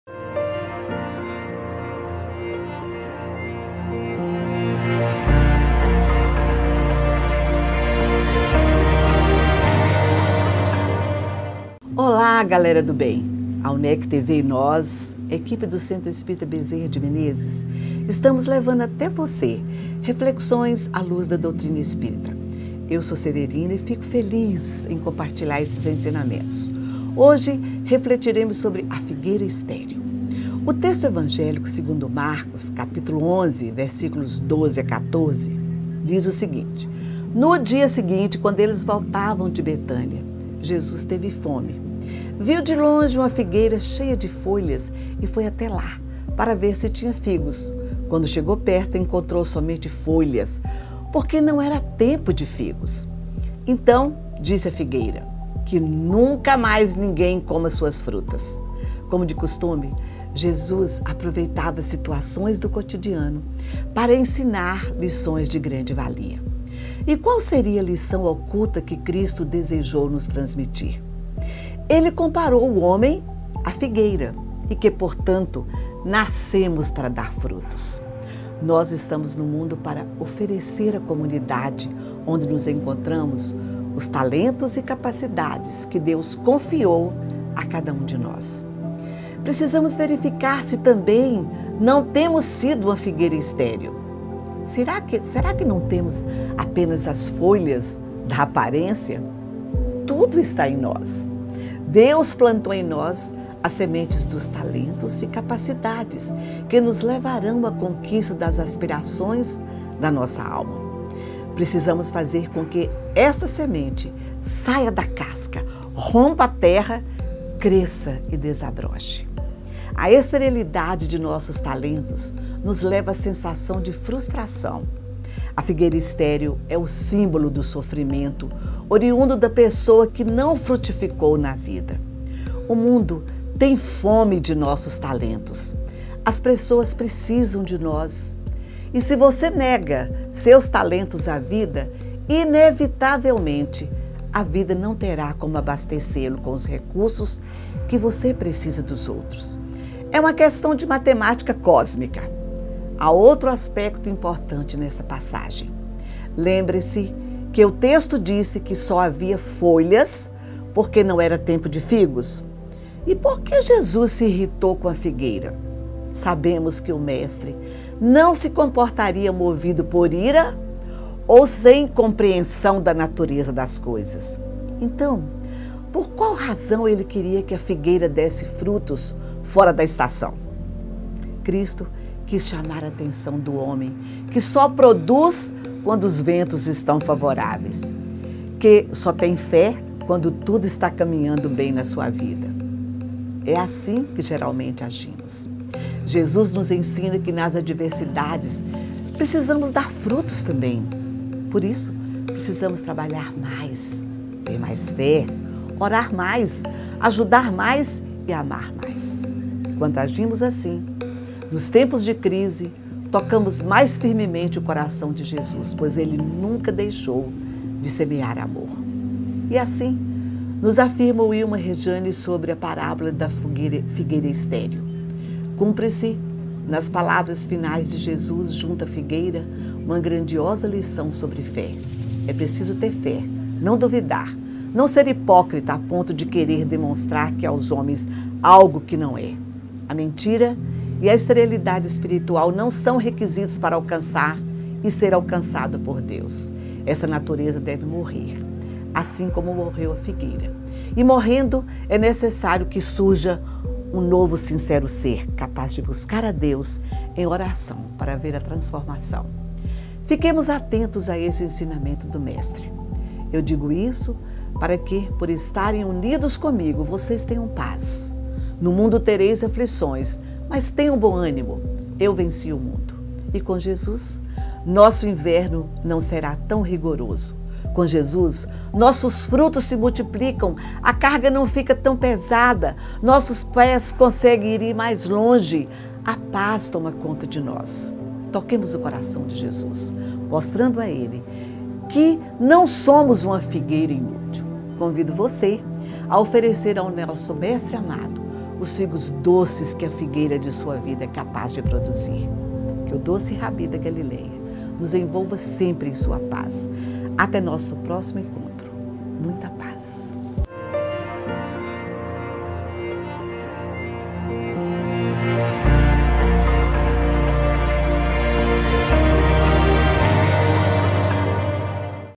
Toque de Fé - Severina de Lima | A FIGUEIRA ESTÉRIL
No Toque de Fé desta semana, Severina de Lima faz uma reflexão sobre nascermos para dar frutos.